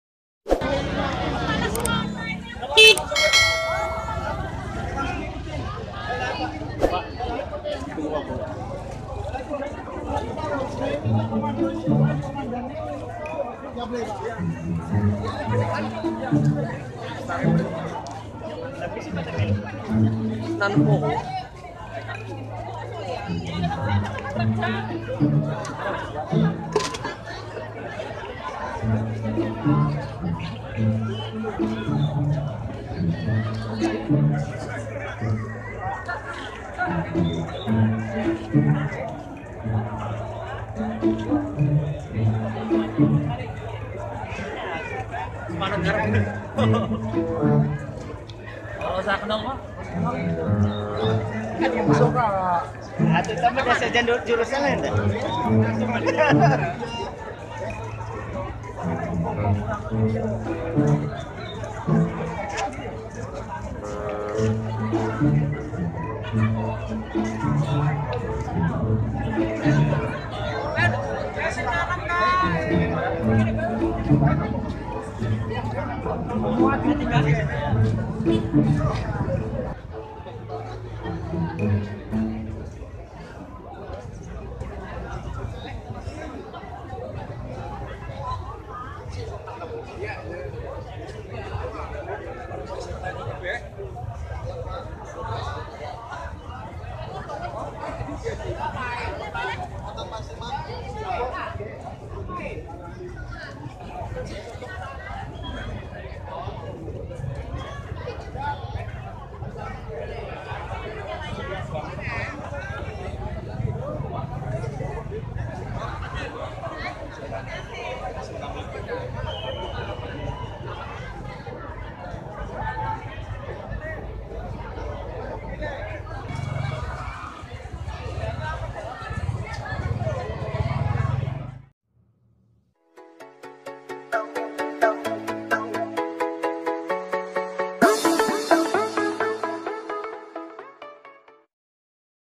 Gerak Jalan Hari Jadi Kabupaten Brebes part 1
Gerak Jalan Hari Jadi Kabupaten Brebes yang ke 342 di kecamatan Ketanggungan Brebes